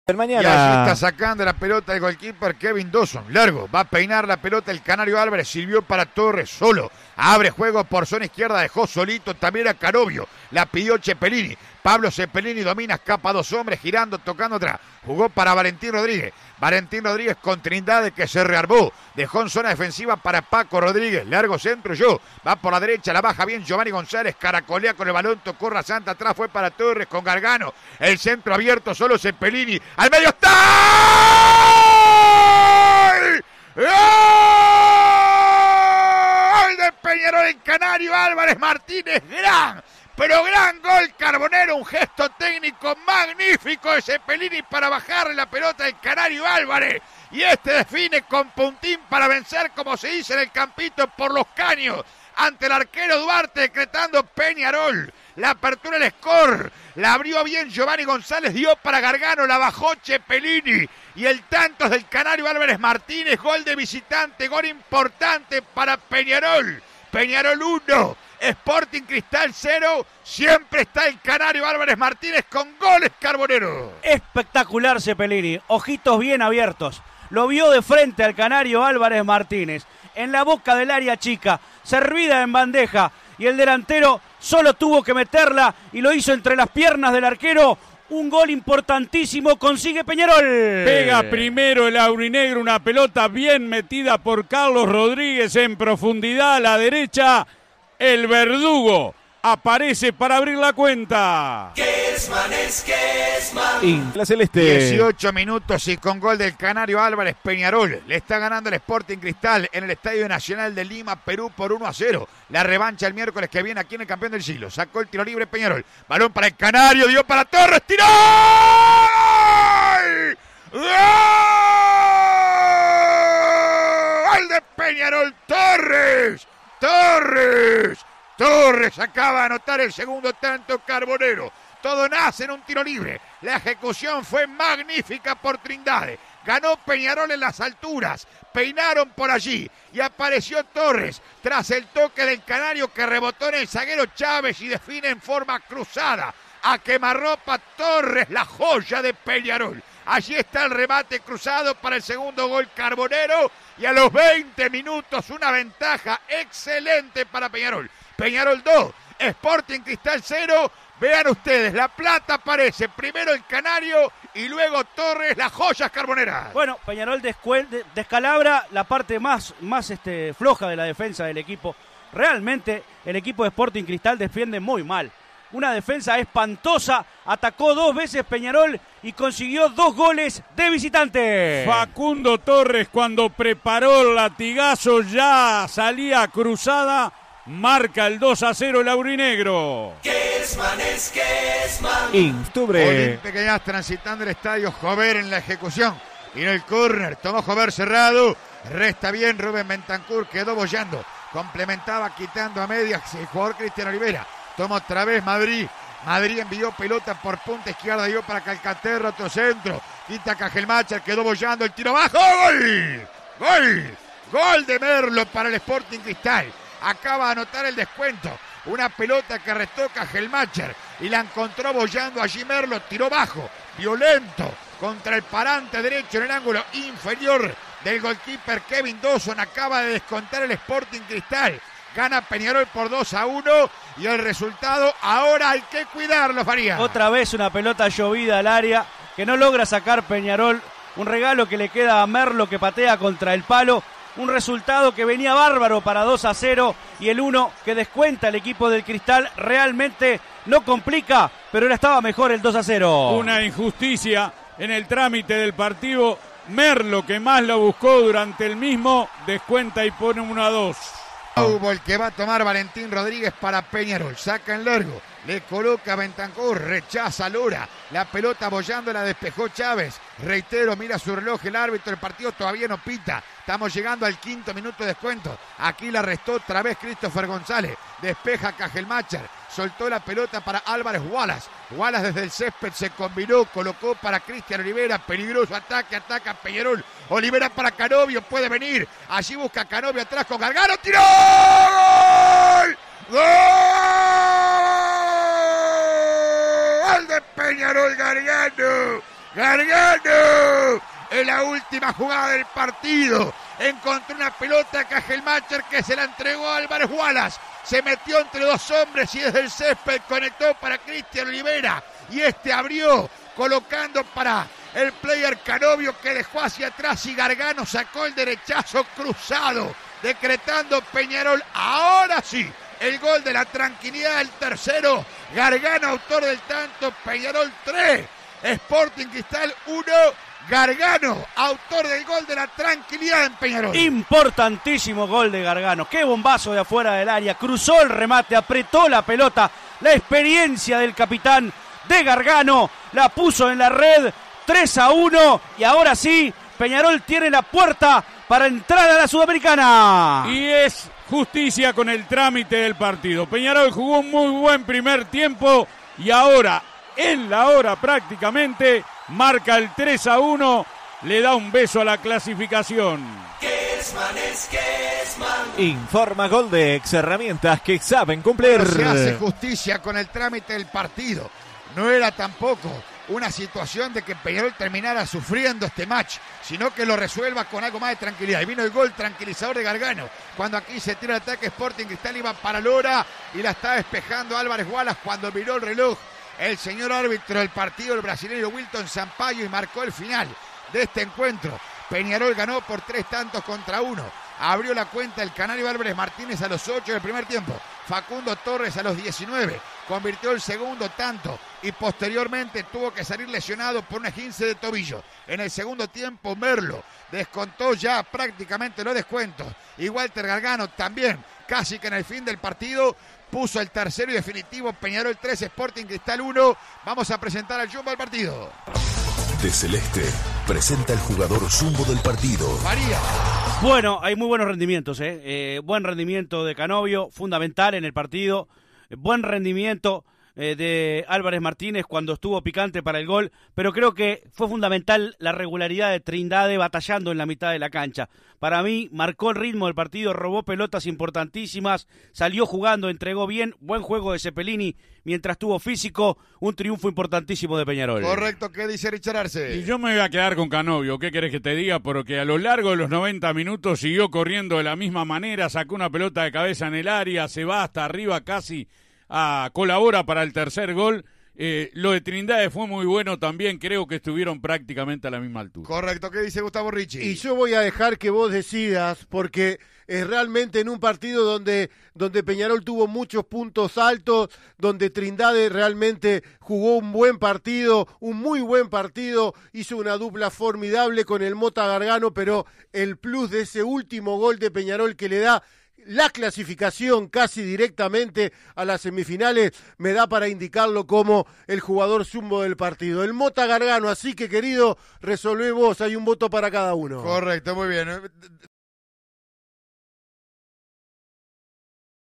Sporting Cristal 1 Peñarol 3 - Sudamericana 2021 (por Martín Kesman)
Los goles del partido, por Martín Kesman junto a Ernesto Faría y Richard Arce para CX 22 Radio Universal 970 AM.